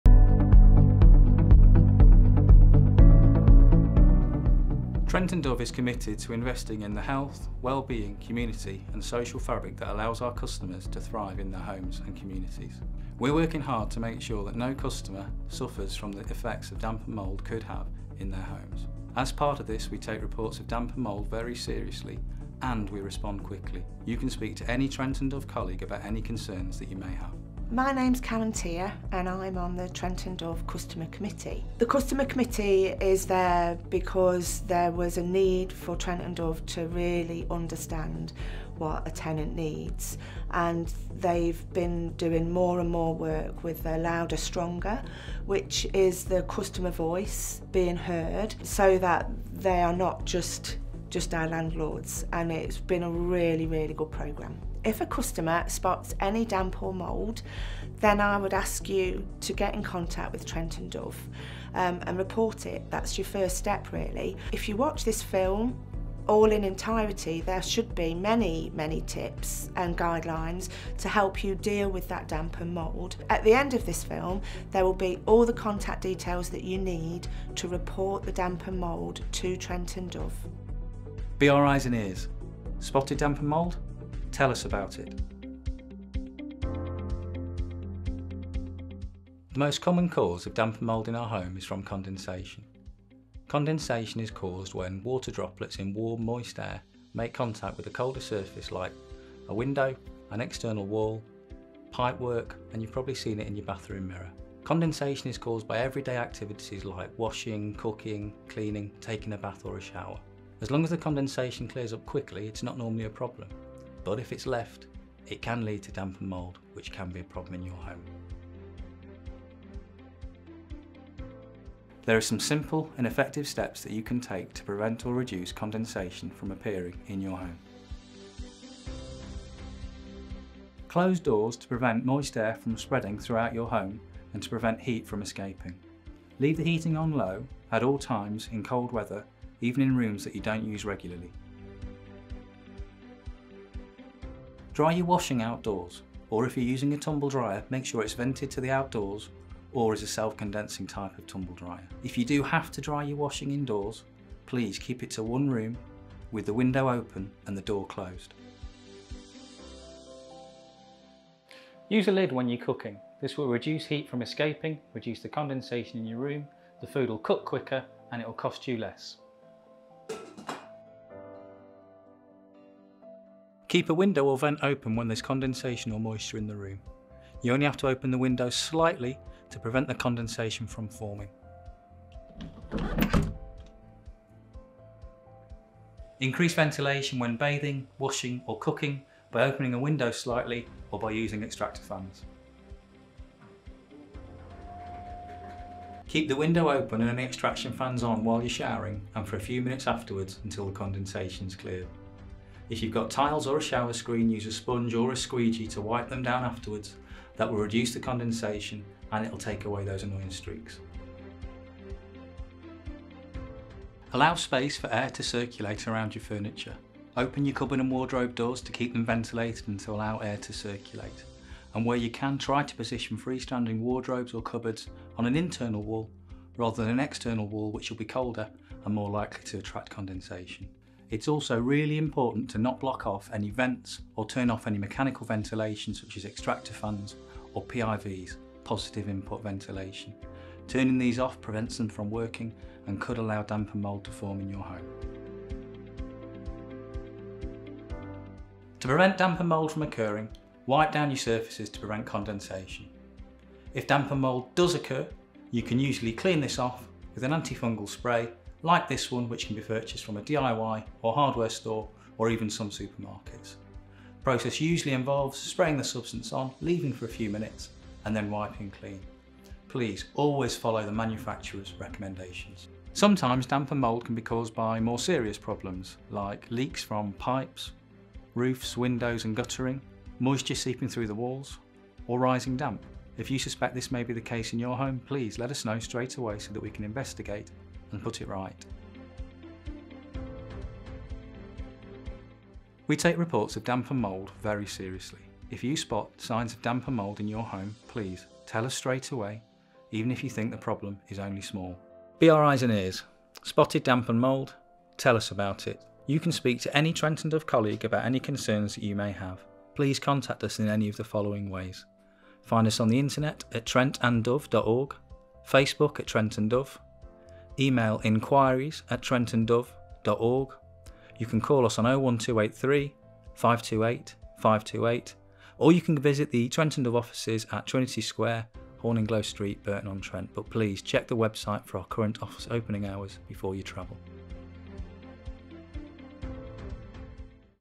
Dealing with Damp & Mould - Trent & Dove Housing.
Advice on reducing the occurrence of damp, mould, and condensation in your home.

Information on how to report damp and mould in your Trent & Dove home, and the support available.

Together we can reduce damp and mould.